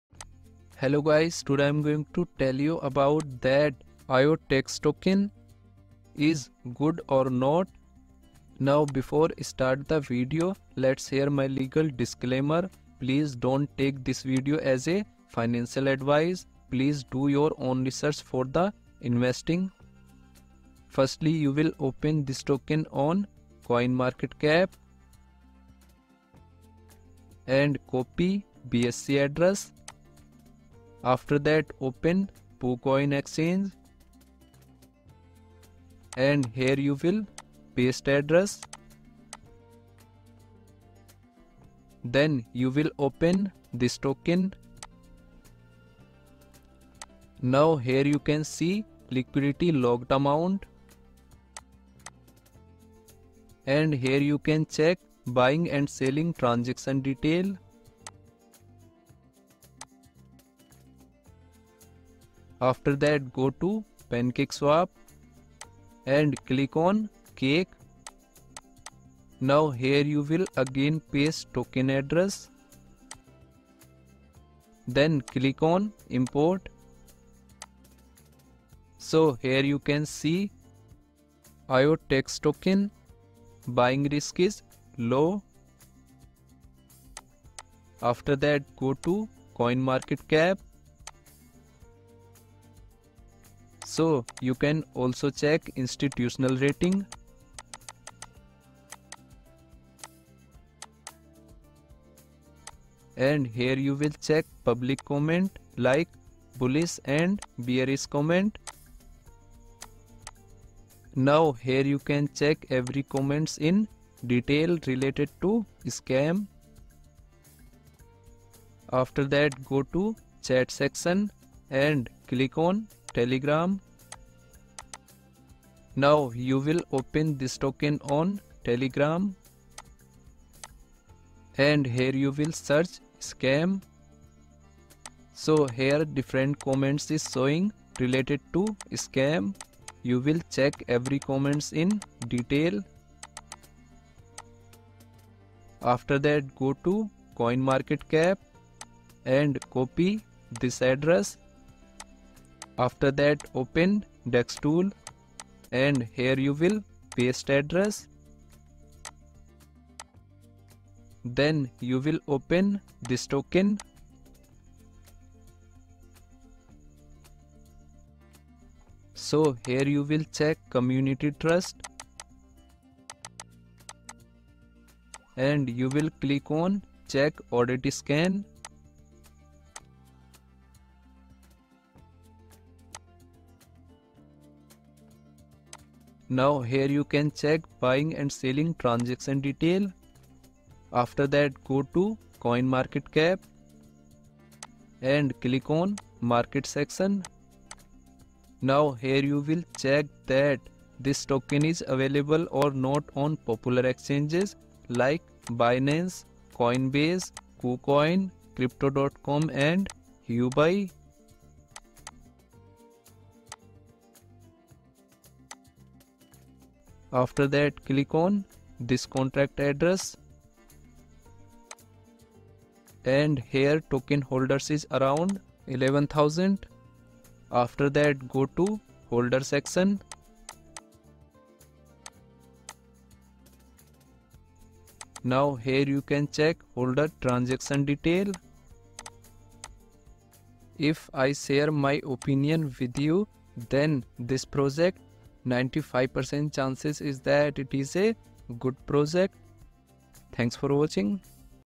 Is IoTeX (IOTX) Token Good Or Not ??
In this video, I am going to share that IoTeX (IOTX) Token is Good or Not. I have made videos based on online data, I am not a financial advisor investing in crypto at my own risk.